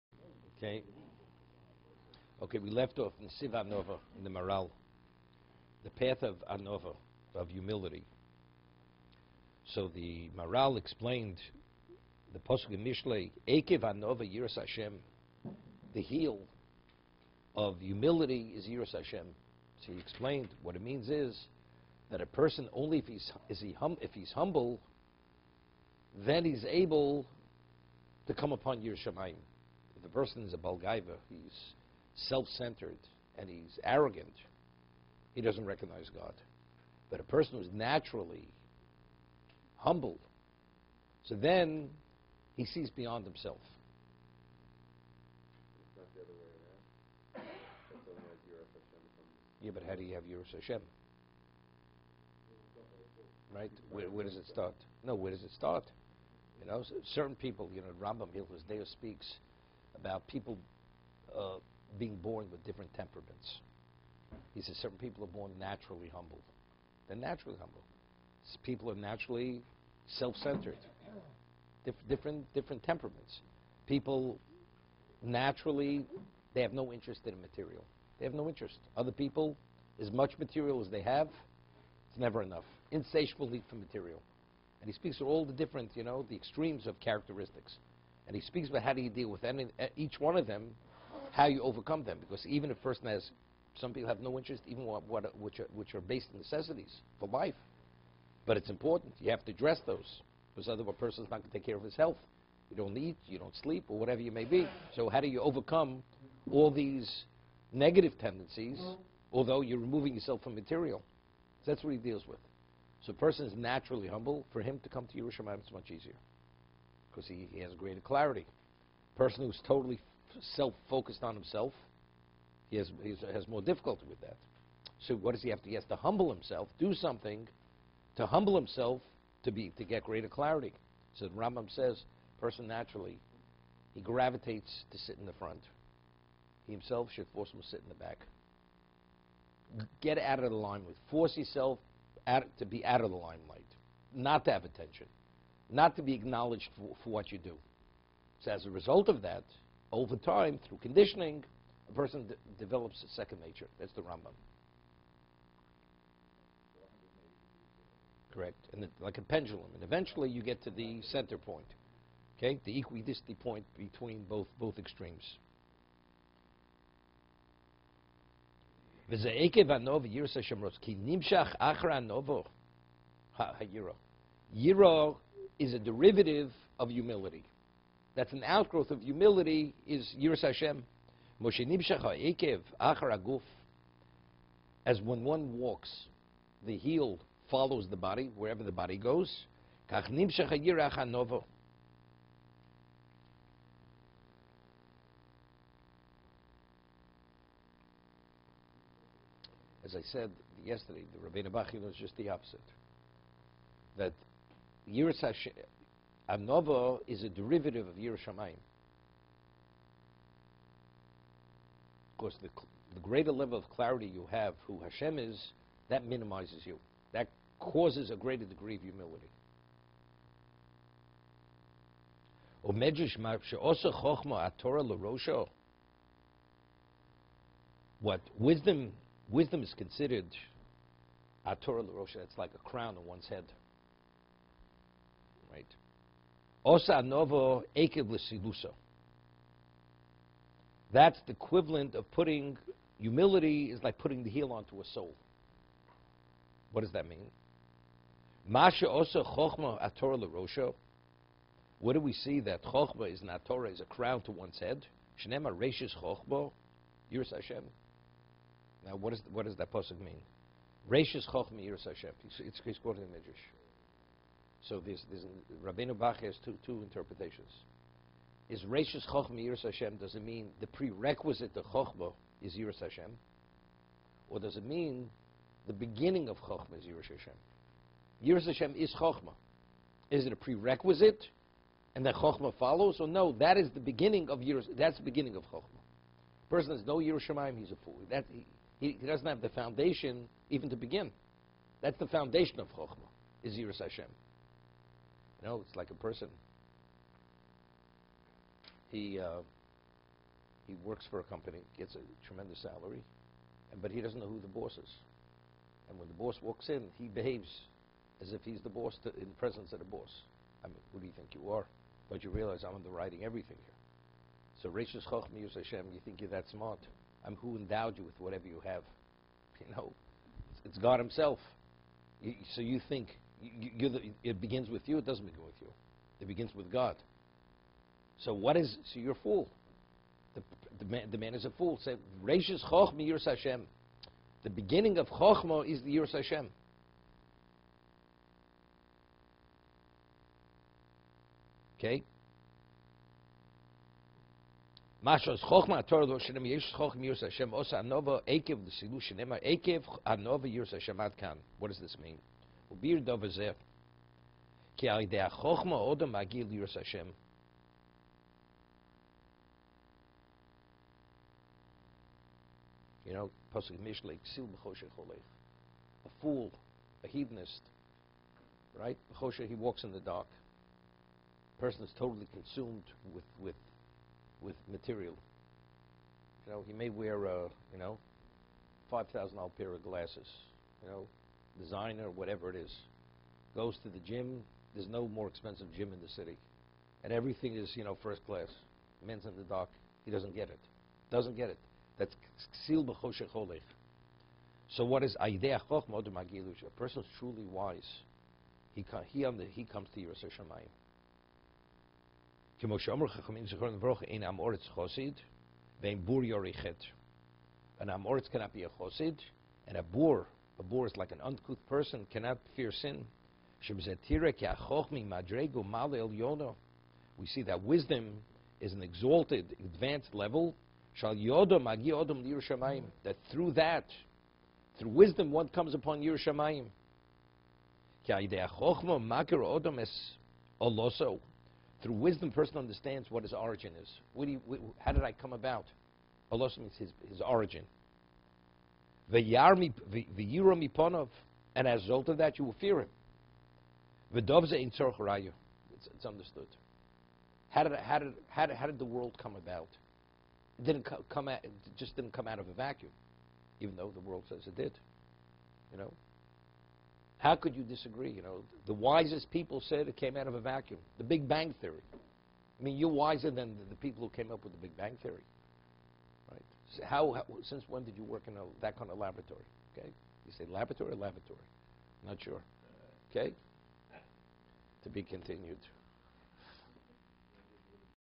An Appreciation of Torah Study lesson 2 Parshas Vayakhel Pekudei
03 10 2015 Tuesday Rabbi Kalatsky gives a daily shiur in depth class on Nesivos Olam which Nesivos Olam, the Maharal, culling numerous sources in the Talmud and Midrash, deals with the profound relationship between the Jewish nation and the Torah,